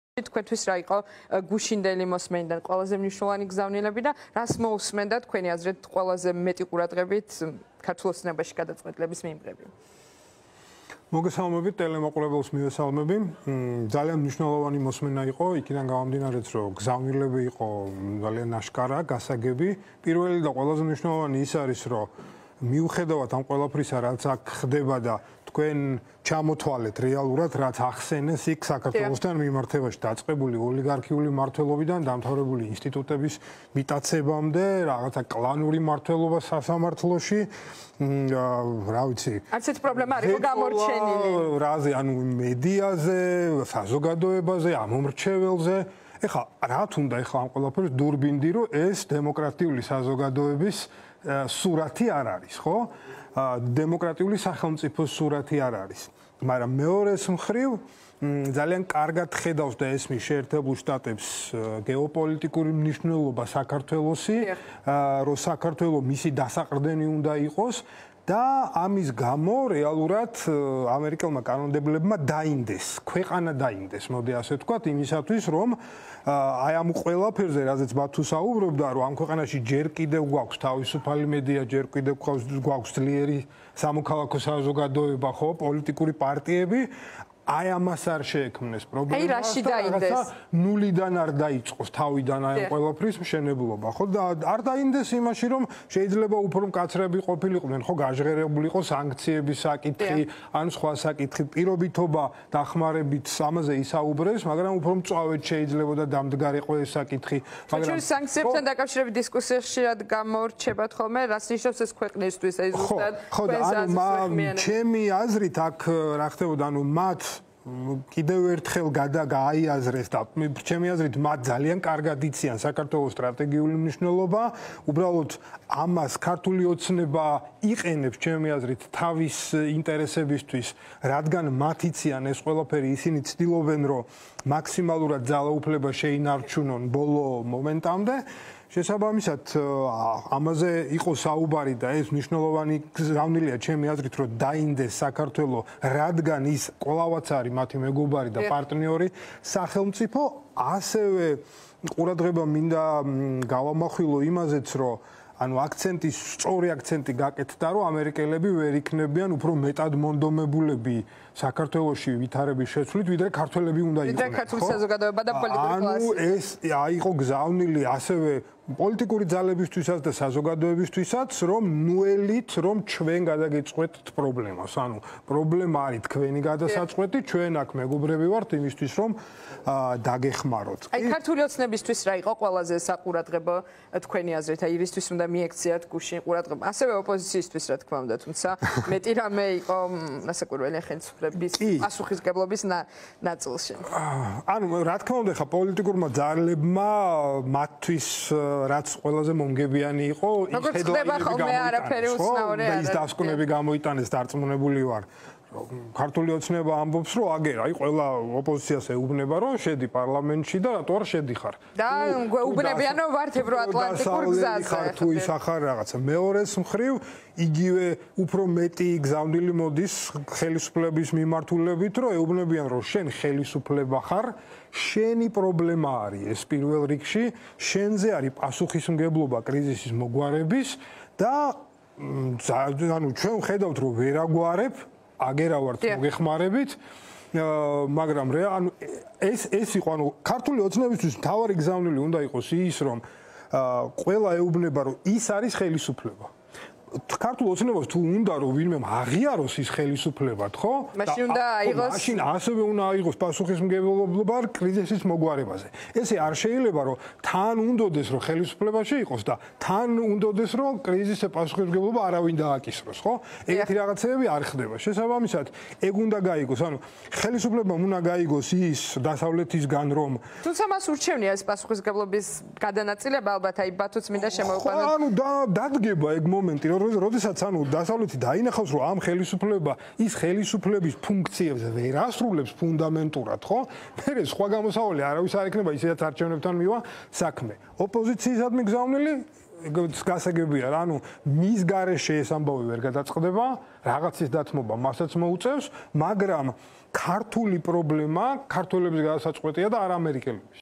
გიორგი რუხაძე | კომენტარი
▪️ „როდესაც დასავლეთი დაინახავს, რომ ეს ხელისუფლება ხელისუფლების ფუნდამენტურ ფუნქციებს ვერ ასრულებს, მერე ისედაც არჩევნებთან მივა საქმე," - გიორგი რუხაძე